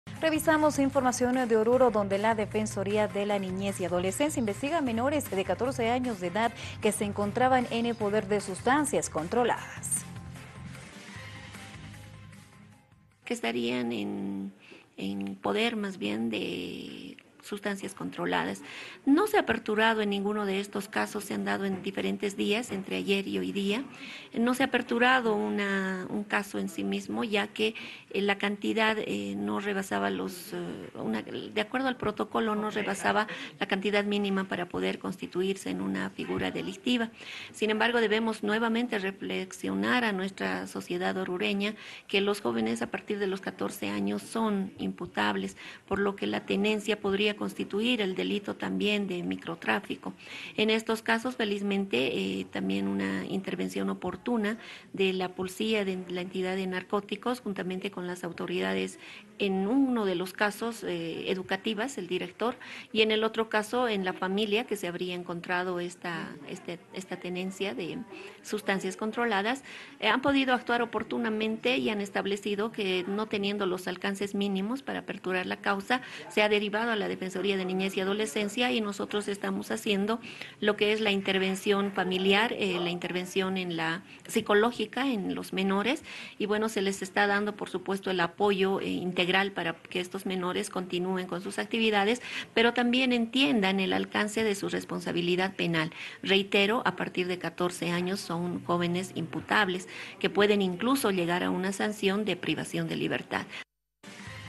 Oruro: Menores son investigados por portar sustancias controladas
Carmen Miranda, directora de la Dirección de Igualdad de Género y Oportunidades de Oruro, informó que menores de 14 años fueron hallados en poder de sustancias controladas, por lo que, debido a su corta edad y no pudiendo ser imputables, fueron derivados a la Defensoría de la Niñez y Adolescencia para recibir ayuda psicológica.Miranda explicó que la cantidad que portaban no rebasaba el límite mínimo para constituirse en una figura delictiva.